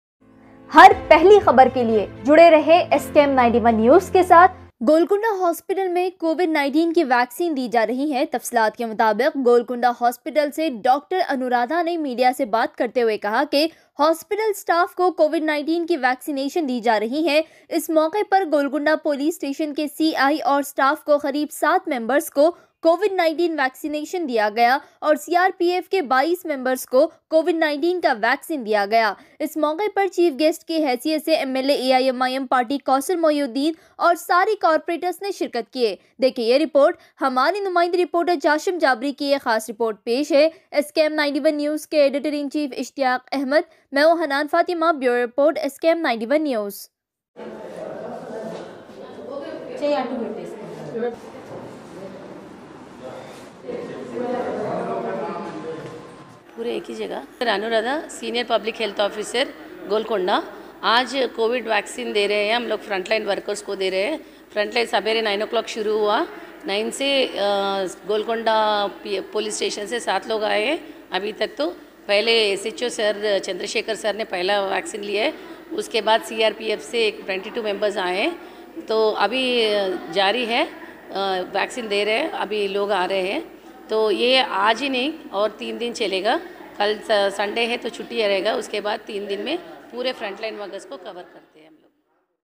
GOLCONDA HOSPITAL MAI HOSPITAL STAFF GOLCONDA PS STAFF 22 MEMBERS KO COVID-19 VACCINE DIYA GAYA
GOLCONDA HOSPITAL MAI COVID-19 KI VACCINE DI JARAHI HAI

TAFSILAT KE MUTABIQ

GOLCONDA HOSPITAL SE DR ANURADHA NE MEDIA SE BAAT KRTEY HUWE KAHA KE .. HOSPITAL STAFF KO COVID-19 KI VACCINATION DI JARAHI HAI, ..

ISS MAUQE PER GOLCONDA POLICE STATION  KE CI AUR STAFF KO KHREEB 7 MEMBES KO COVID-19 VACCINATION DIYA GAYA ..

AUR CRPF KE 22 MEMBERS KO COVID-19 KA VACCINE DIYA AYA ..

ISS MAUQE PER CHIEF GUEST KI HASIYAT SE MLA AIMIM PARTY KAUSAR MOHIUDDIN AUR SAREY CORPORATERS NE SHIRKAT KIYE

DEKHE YEH REPORT

HUMAREY NUMAINDE REPORTER JASHIM JABRI KI KHASS REPORT PESH HAI